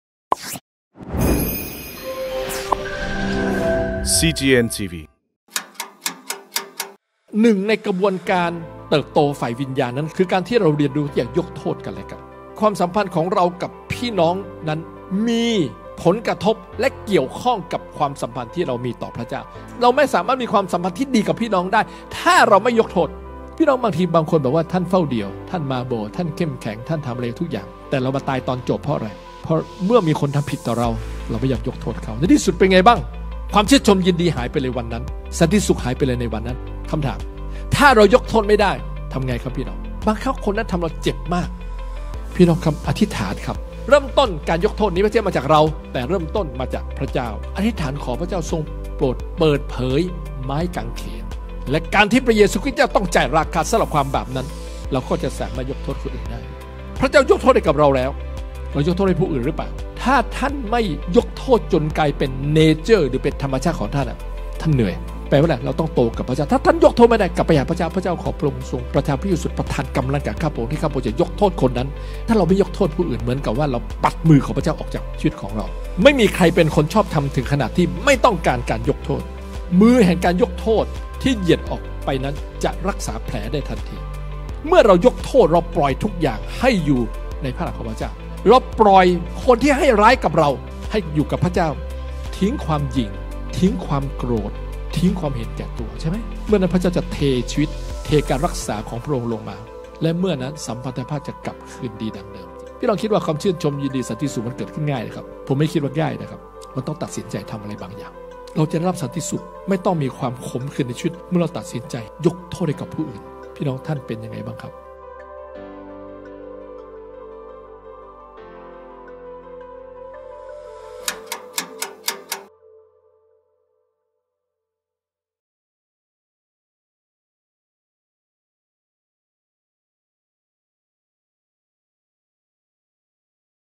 ทำไมต้องยกโทษ? | Just A Minute EP 37
"เมื่อเราไม่ยกโทษให้คนอื่น
ก็เหมือนการที่เรากำลังปัดมือของพระเจ้าออกจากชีวิต"

ตลอดทั้งปีที่ผ่านมา คุณมีความขมขื่นจากการถูกใครบางคนทำร้ายกลายเป็นแผลที่เจ็บปวด จนทำให้ใช้ชีวิตแบบไม่มีความสุขรึเปล่า?

มาเริ่มต้นใหม่ ขอการช่วยเหลือจากพระเจ้า
และมาเรียนรู้จักการ 'ปล่อยมือจากความเจ็บปวด' เพื่อมีความสุขกัน 😊